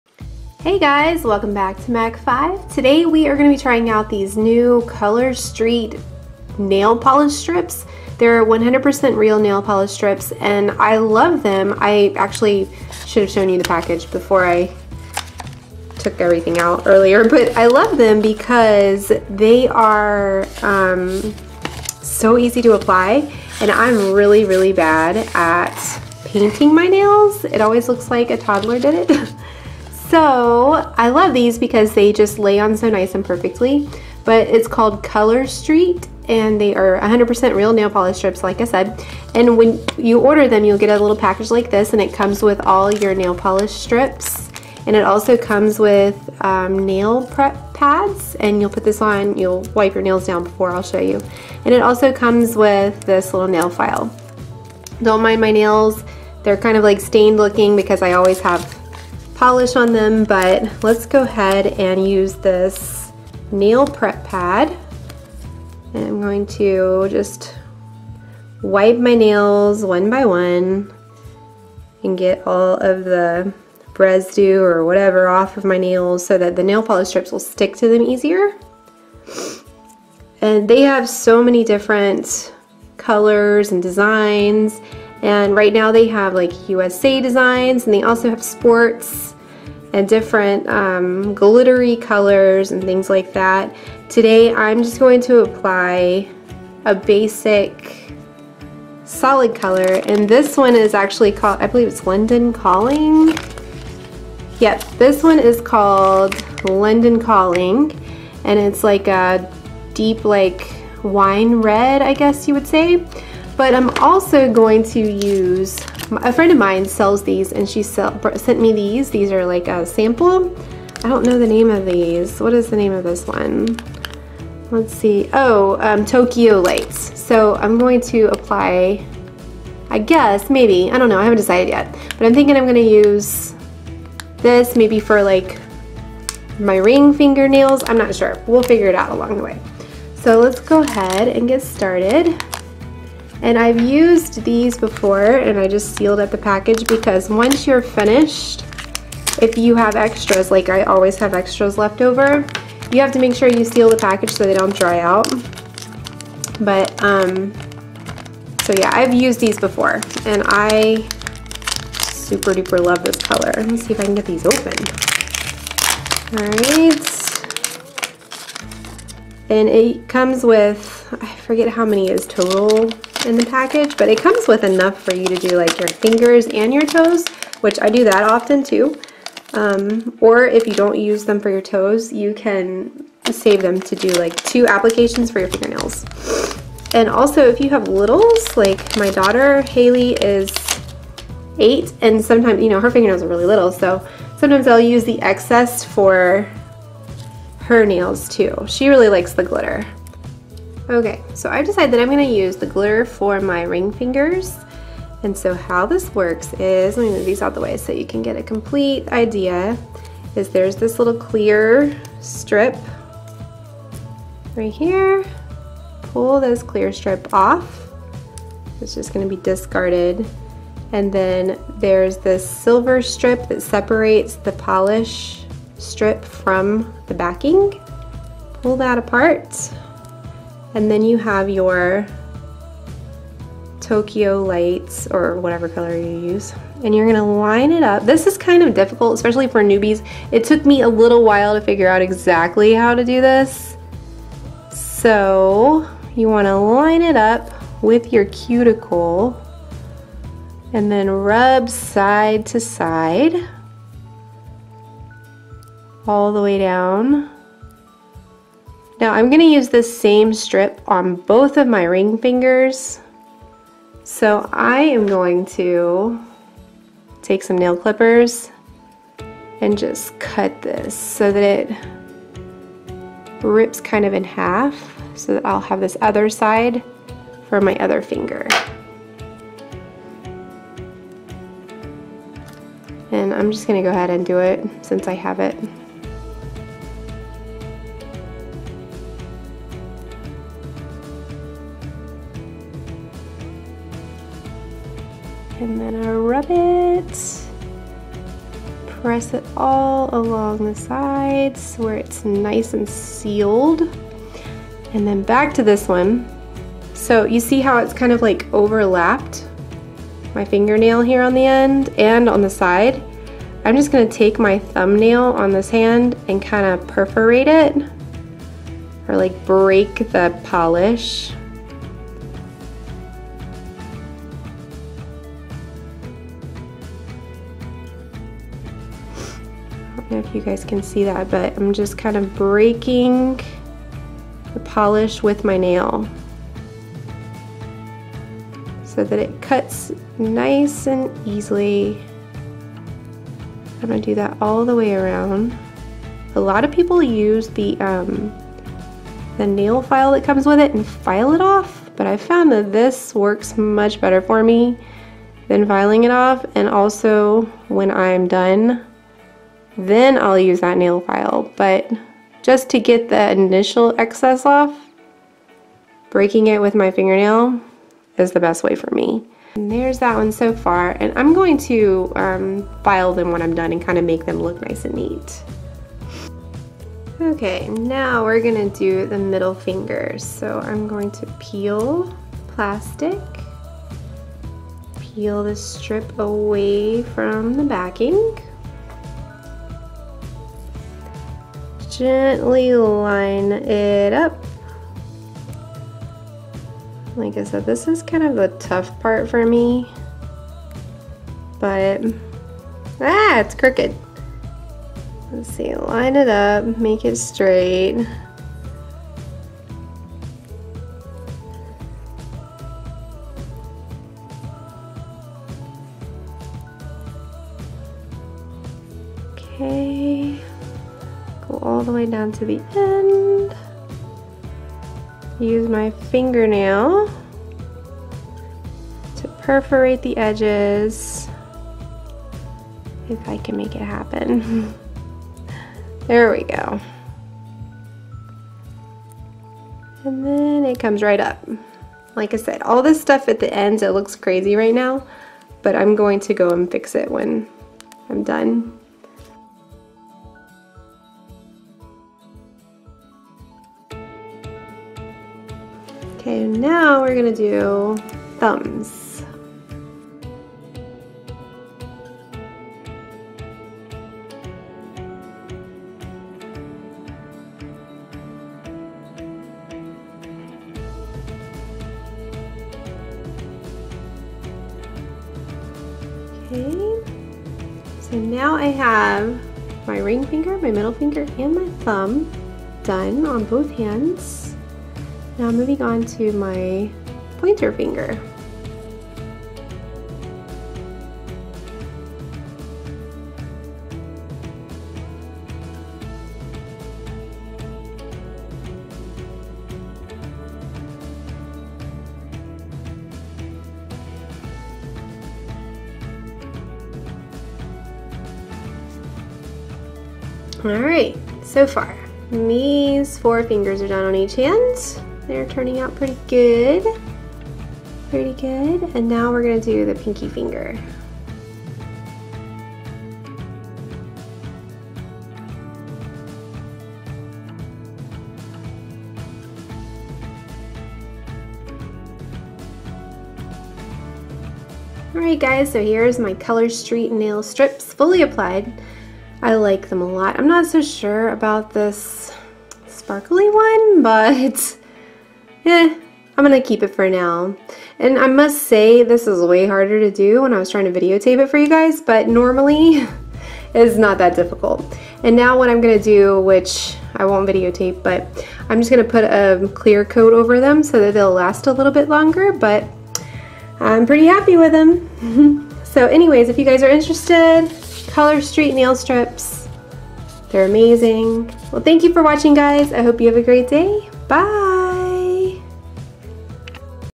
Color Street Nail Polish Strips Review!
Applying and Reviewing Color Street 100% Real Nail Polish Strips! I applied the London Calling and Tokyo Lights colorstreet strips today and I love them!! With Color Street, no need to wait for your nails to dry. Just apply and go! They are awesome!